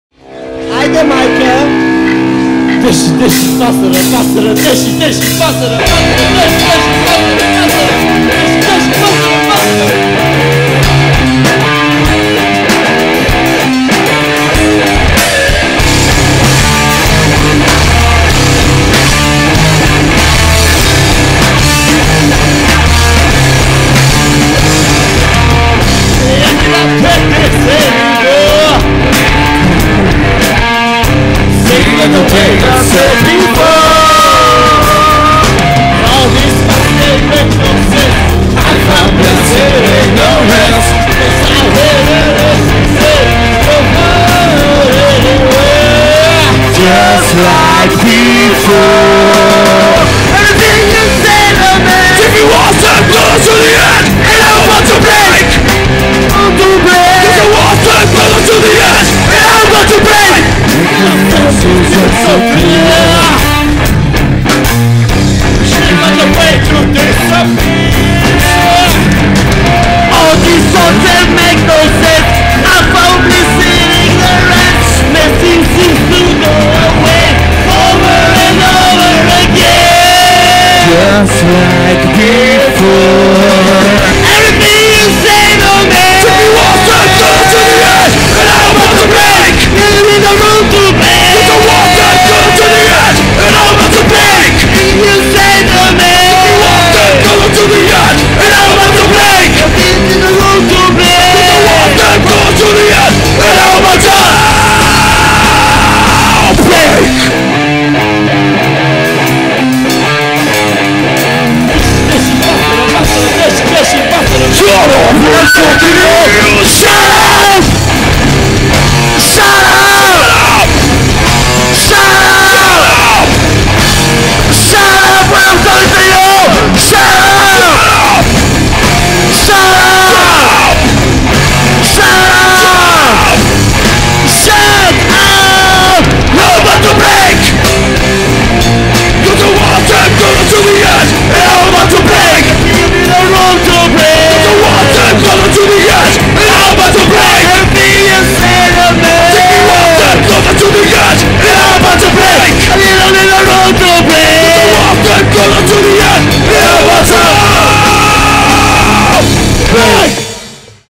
Beck'stage 2012 -- Камен Каменов & Павел Хаджиев
Beck'stage at Spirit of Burgas 2012, Bulgaria - 224&224А